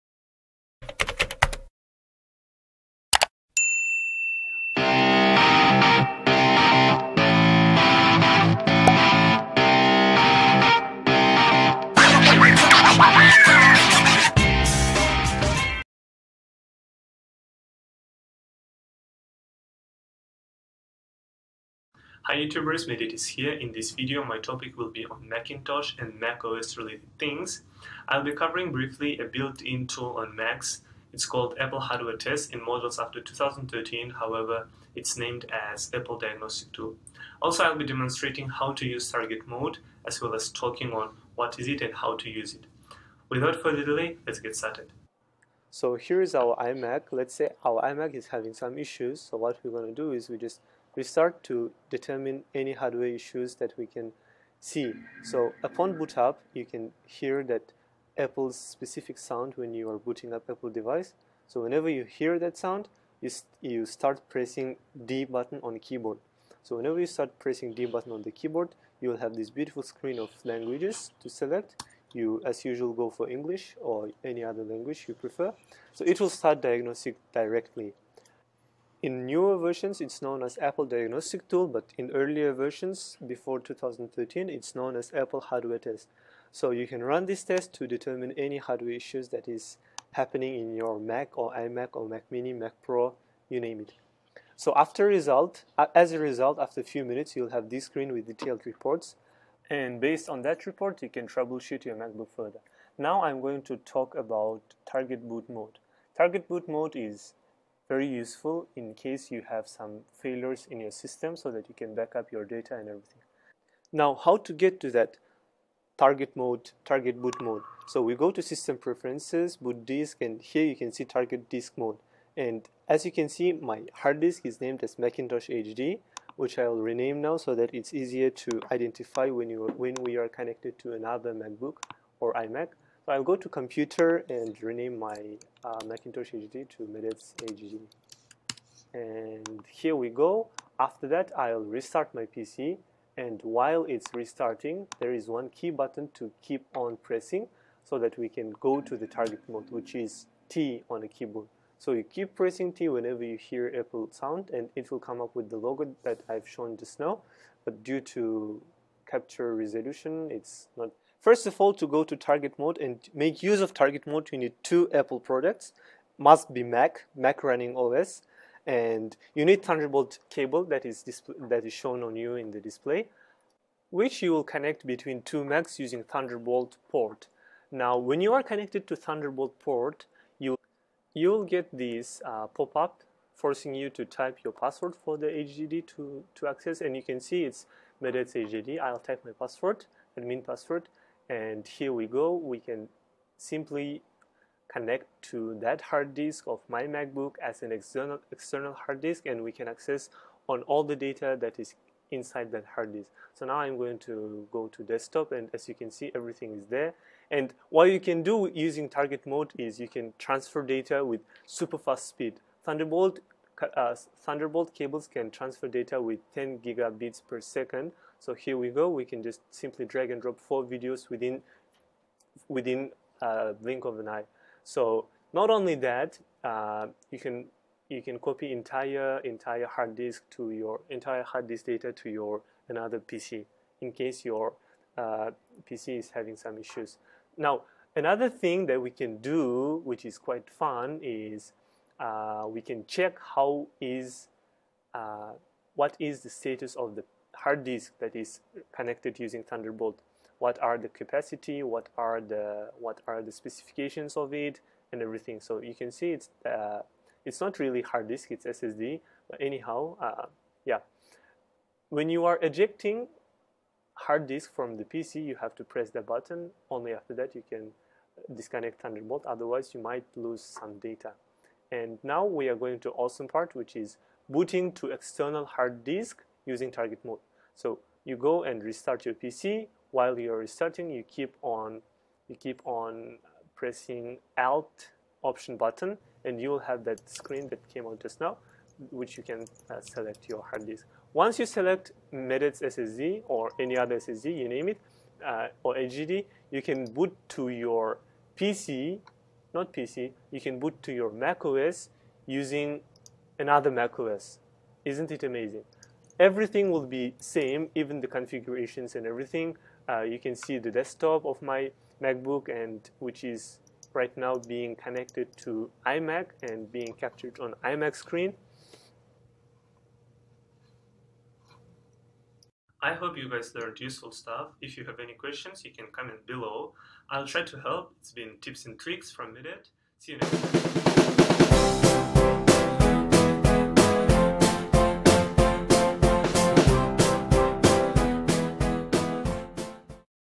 Tips & Tricks#60 How To Use Apple Diagnostic Tool & Target Boot Mode
Prepared by Medet,

In case your Mac is acting weird, there are few basic and essential things that you can do to try and solve it!
In this week's Tips and Tricks, Apple Diagnostic Tool (also known as Apple Hardware Test) will be demonstrated.
Moreover, awesome and exclusive feature named Target Boot Mode will be explained and demo-ed!